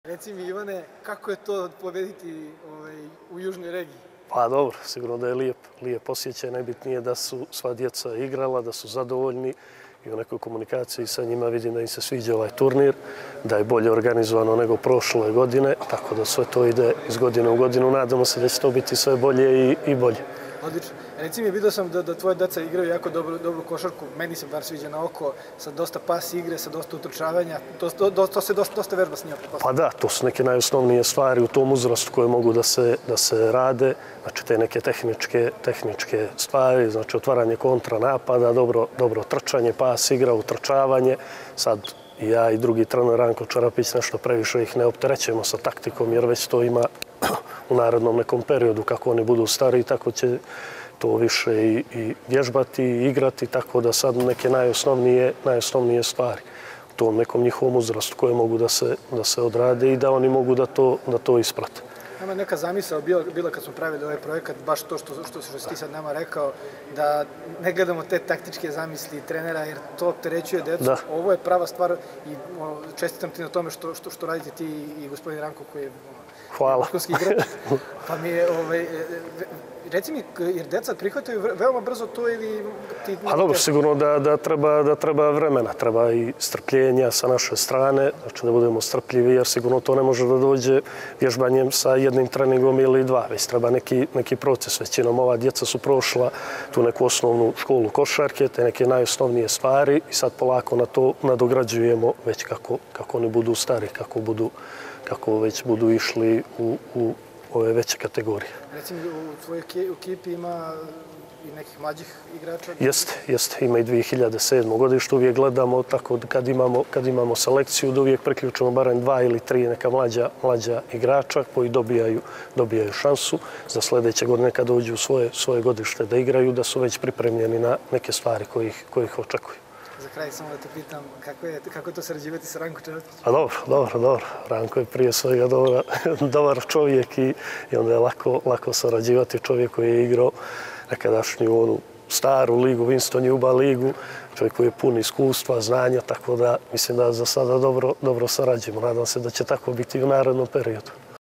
BASKET 4 KIDS | Ivan Raković Trener KK Mediteran | Basket4Kids Bar
pratite nas na našem novom kanalu ➜ Vesti i informacije pratite na Sport Klub sajtu

Pogledajte izjavu trenera KK Mediteran, Ivana Rakovića, koju je dao ekskluzivno za Sport Klub tokom turnira Basket4Kids u Baru.

Na turnirima „Basket 4 Kids“ učestvuju dečaci iz svih zemalja bivše Jugoslavije – Srbije, Hrvatske, Slovenije, Crne Gore, Bosne i Hercegovine i Makedonije. Ove godine, najstariji učesnici su 2005. godišta.

Glavni cilj ovog velikog regionalnog projekta je da se deca ohrabre da komuniciraju i druže se. Ovo je sjajna šansa za klubove koji rade sa mladima da prikažu svoj rad van granica svojih zemalja.